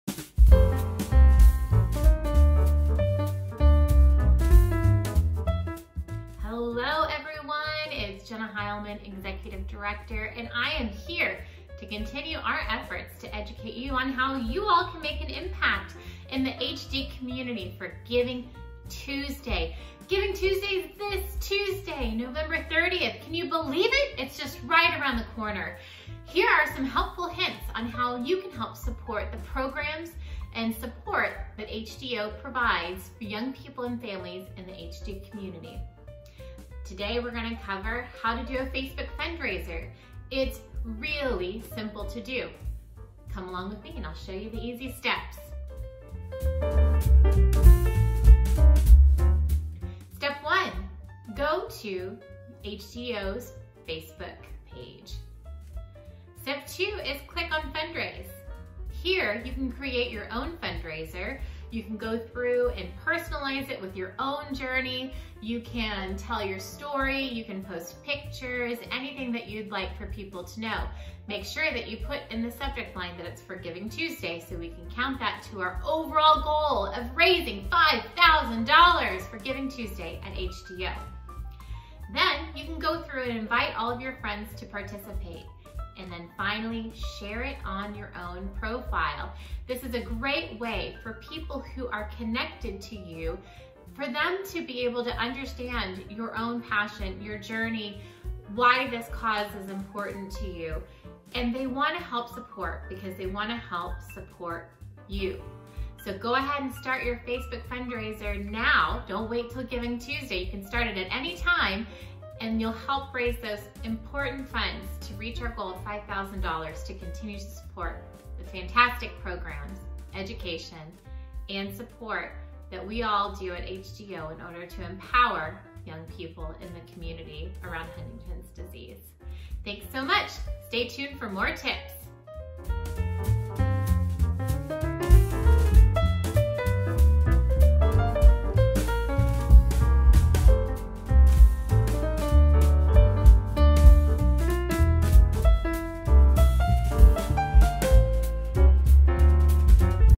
How to Set Up a Facebook Fundraiser for GivingTuesday
HDYO Executive Director, Jenna, shows you how you can setup a Facebook Fundraiser to support HDYO to help raise $5,000!